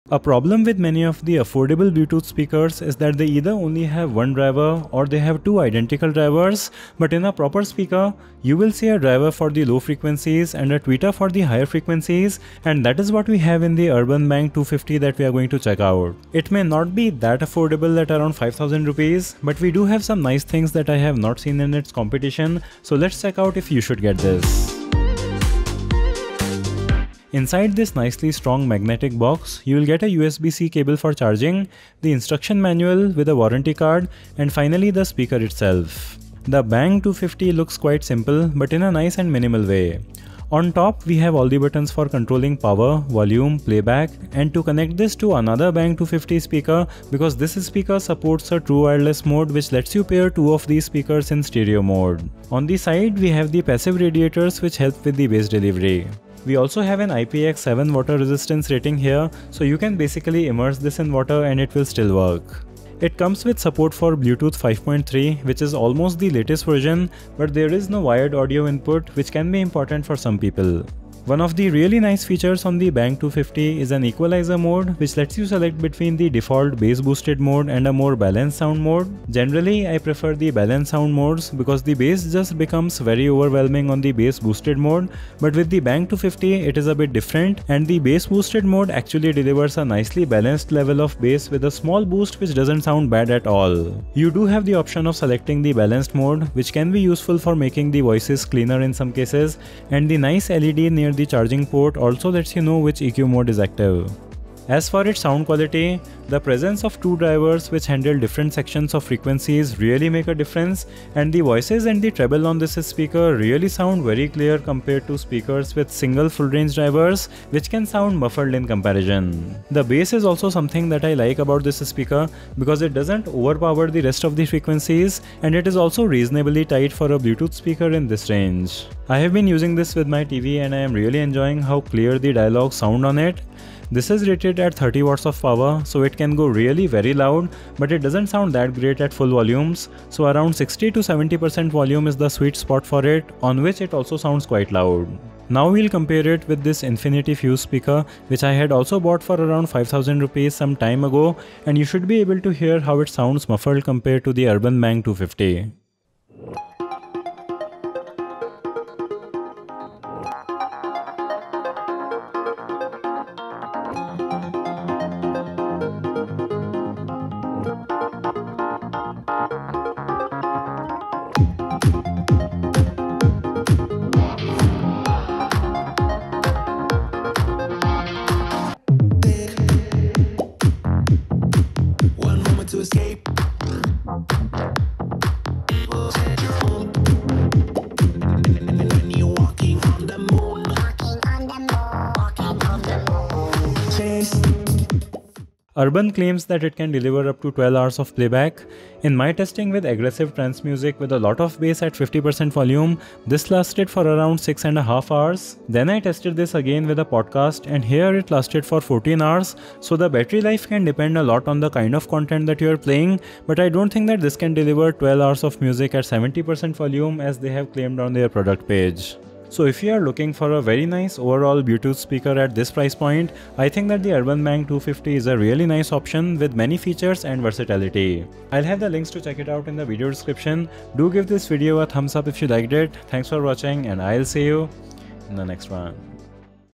Woofer + Tweeter Under ₹5000! URBN Bang 250 Bluetooth Speaker Review!
URBN Bang 250 delivers a great sound with a two way woofer + tweeter driver setup!

⭐ My Recommended Products on Amazon

Chapters:
00:00 - Intro
00:27 - Unboxing
00:36 - Design & Features
01:44 - Sound Quality
02:35 - Sound Test
03:15 - Battery Life & Conclusion